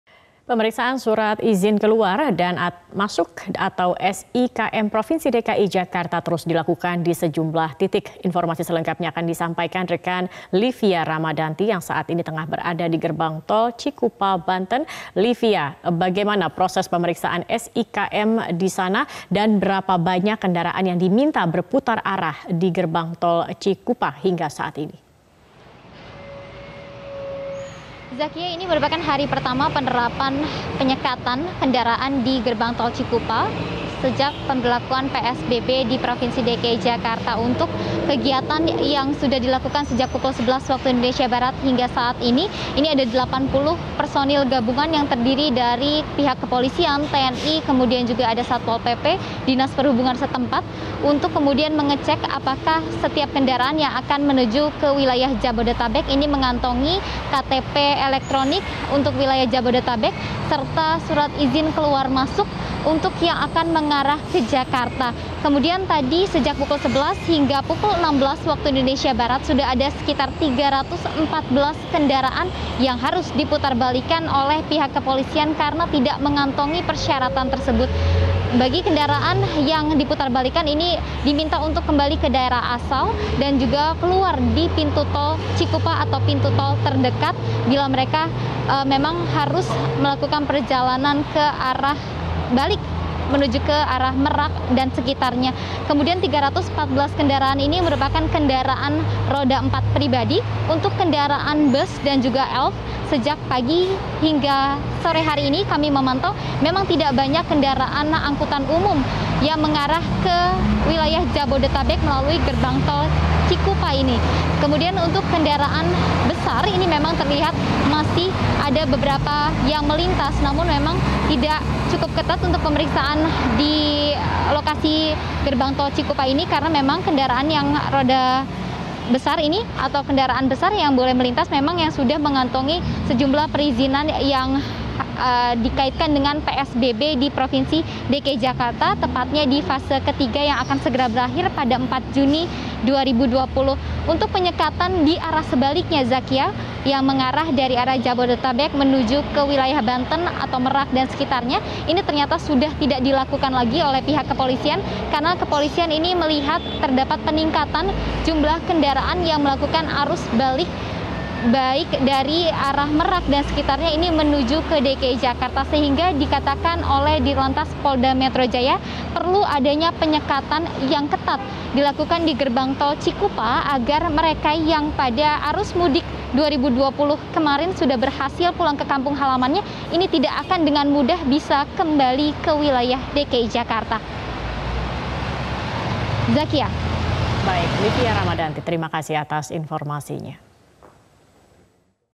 Petugas Gabungan Lakukan Pemeriksaan SIKM di GT Cikupa
Pemeriksaan Surat Izin Keluar Masuk (SIKM) mulai beroperasi di pos penyekatan di gerbang Tol Cikupa, Tangerang. Tercatat hingga sore ini, Rabu (27/5), sudah ada 314 kendaraan dipaksa putar balik menuju daerah asal masing-masing.